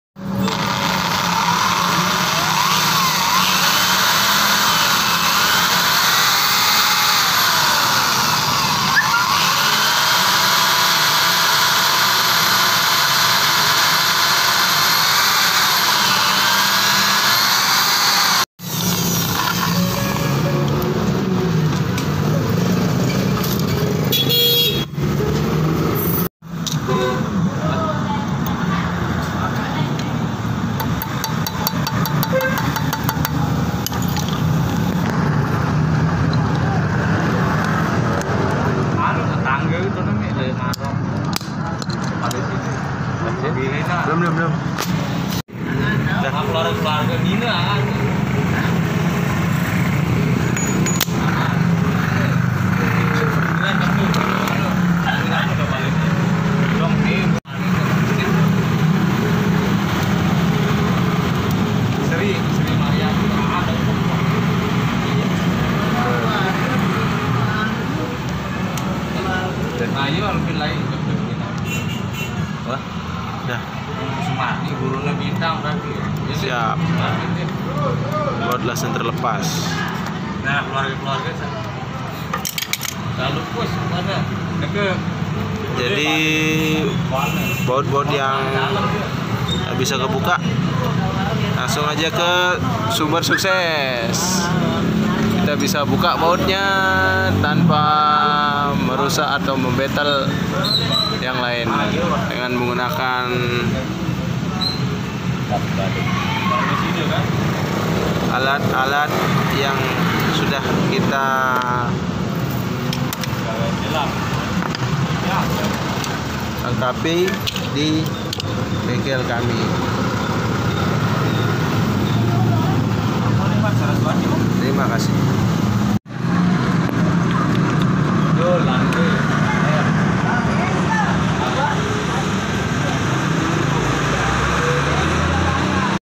Cara membuka baut yang dool mengunakan alat drat kebalik
service servicemotor bengkellombok motorcycle Hati2 saat membuka baut jika kunci yang tidak pas / tidak sesuai maka baut yang ingin di buka malah mata bautnya jadi rusak / dool ., jadi harus di pelajari dulu mengunakan obeng yang sesuai kebutuhannya .